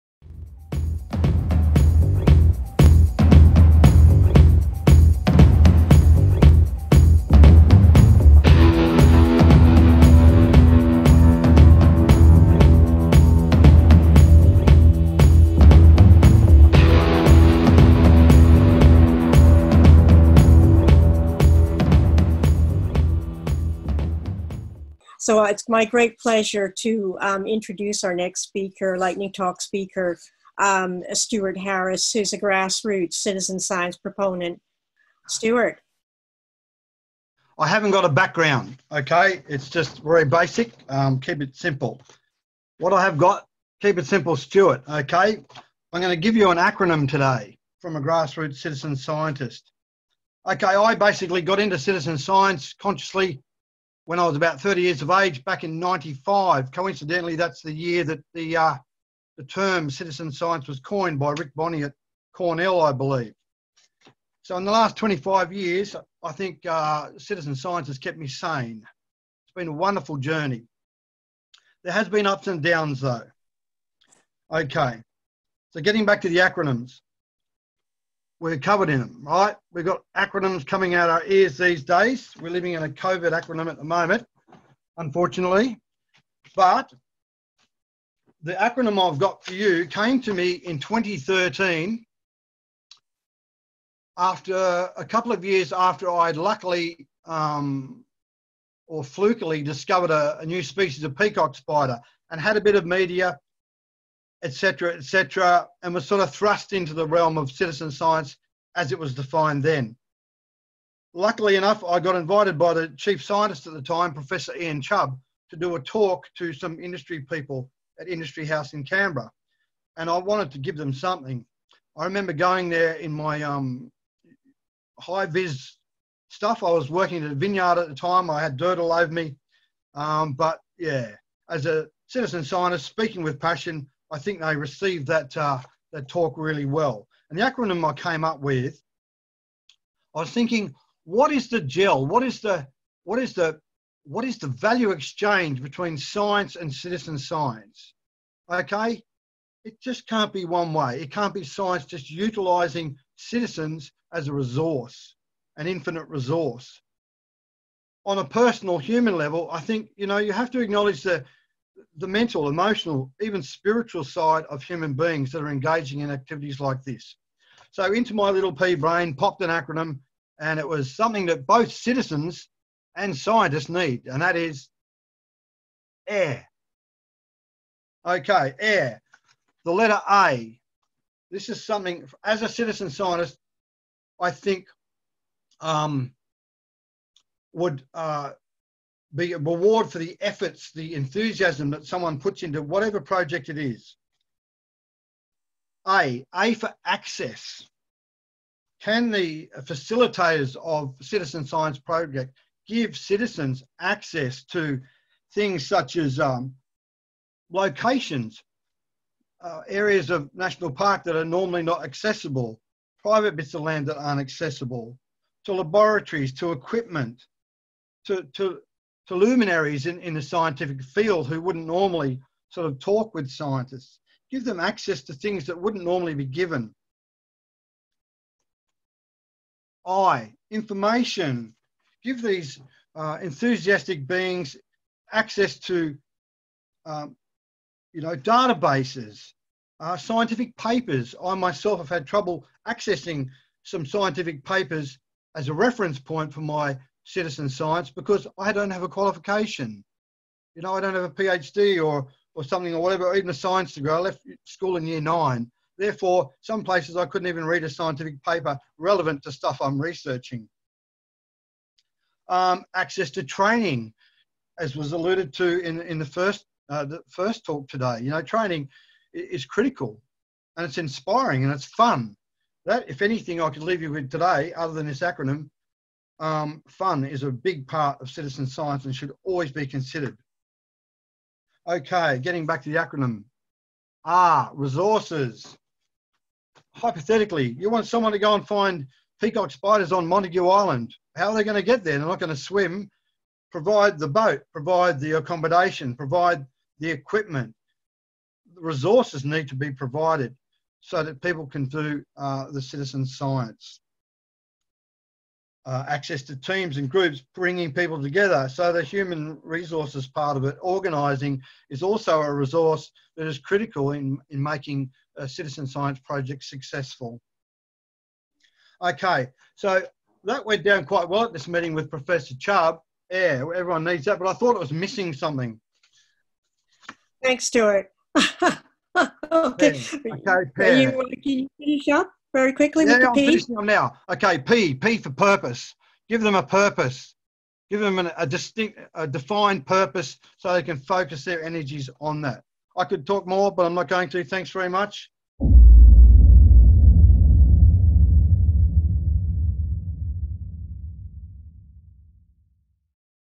CitSciOzOnline EMCR: PAIRing Science and Citizen Science - Stuart Harris
PAIRing Science and Citizen Science

Citizen Science (CS) is a relatively new term for an age-old pastime. Since this term was coined (1995) the world has changed drastically, especially in regard to climate change and world economies. More pressure is on Science to come up with the answers yet less funding and resources are being allocated to it. Costly fieldwork, the mainstay of science, is on the decline though this is where CS may be able to help; at what cost? There is no pay involved with employing the broad resource of CS where its ‘capital’ is primarily a purely human endeavor. How do we reward this human effort with particular emphasis on the emotional, mental, and even spiritual planes!?

Stuart Harris is a grassroots citizen science proponent who came to notice after accidentally discovering a new species of peacock spider back in 2008. He cut his teeth in CS as a bird watcher back in the mid-90s, gave ten years to spider observations, and recently has entered the realm and culture of Coleoptera, primarily the observation and recording of jewel beetles. Stuart has led many BioBlitz and birding groups and is a strong advocate for CS where he has been interviewed widely and has given talks to a broad range of audiences. Stuart is currently employed by Questacon and is a field technician specialising in Superb parrot conservation and ecology. He also volunteers at the Australian National Insect Collection at CSIRO Black Mountain. A keen photographer and cyclist, Stuart lives with his wife Jeab in Canberra.